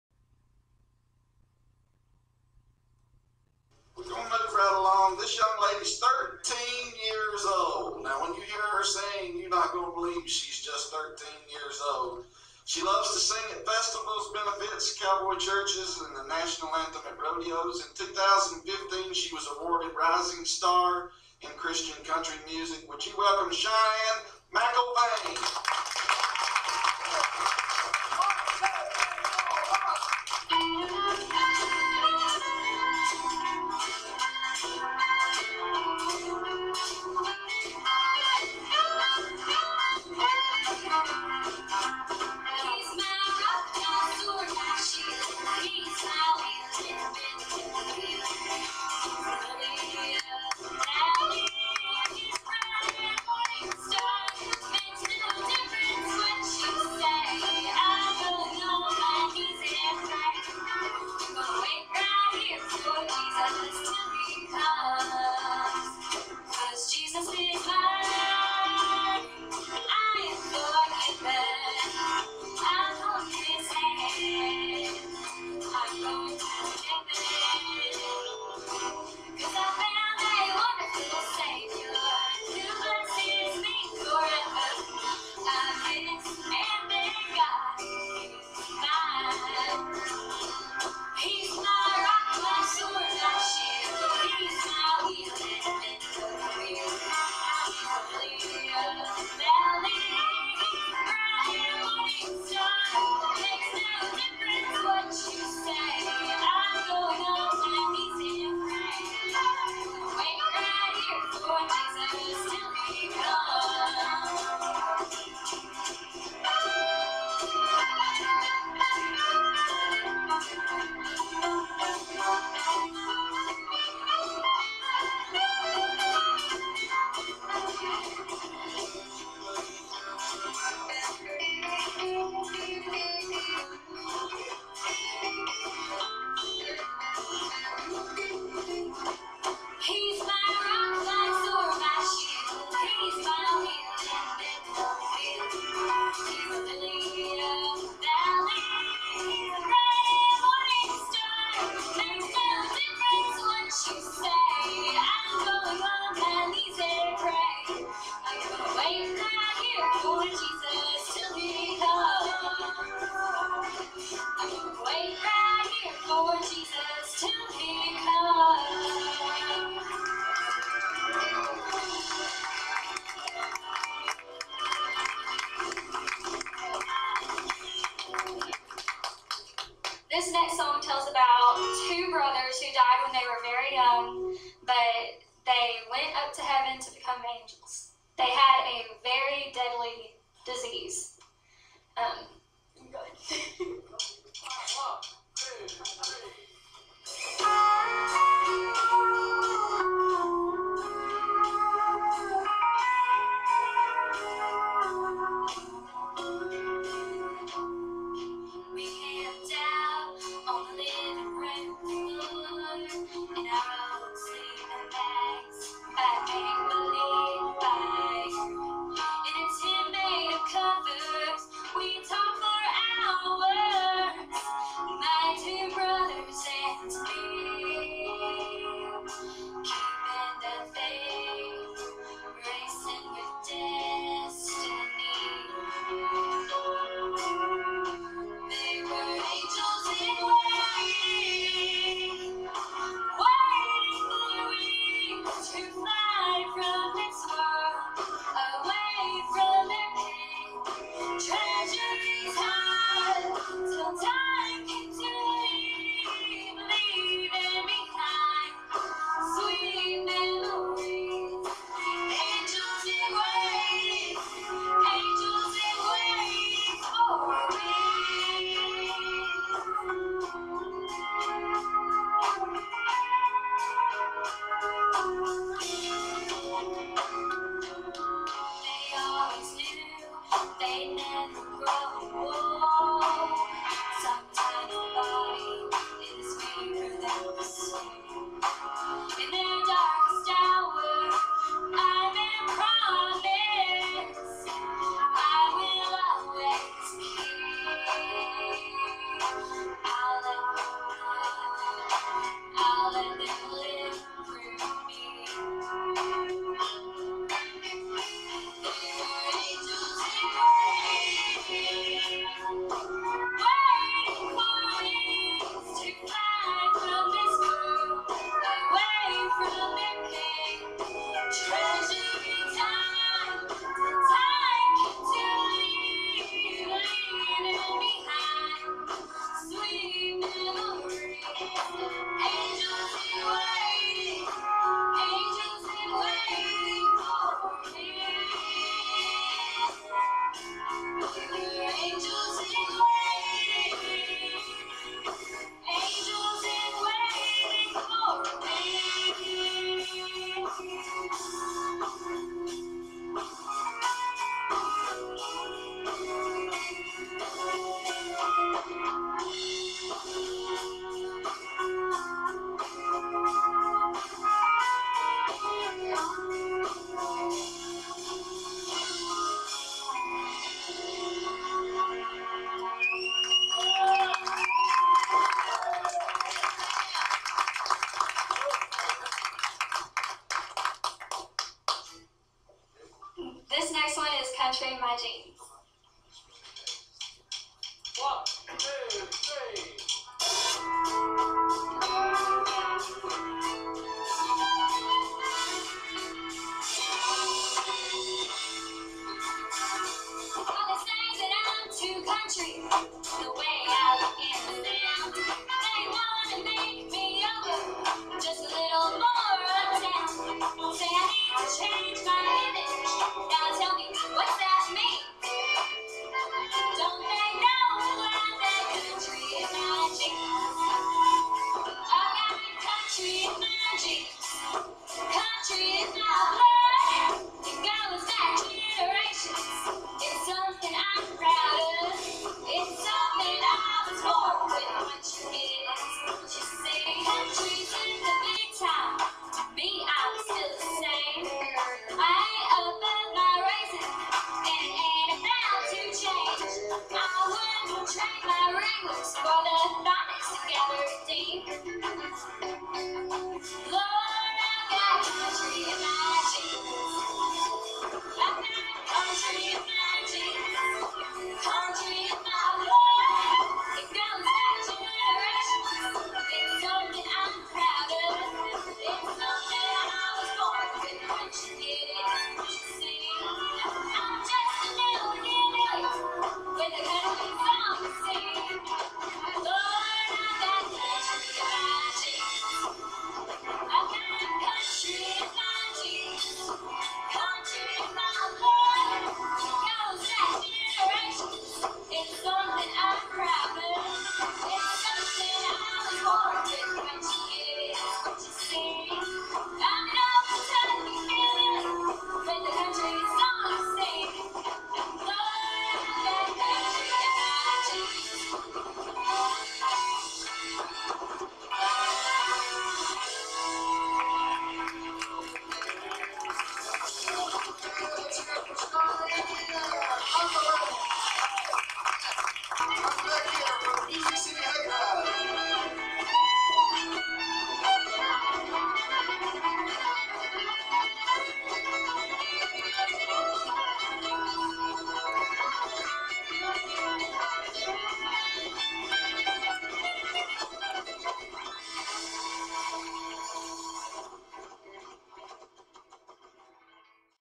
Cheyenne Singing at Music City Hayride Show
Cheyenne McIlvain performing in Nashville, TN at the Music City Hayride Show 2016
Special Thanks to Mai Larsen for recording this amazing video.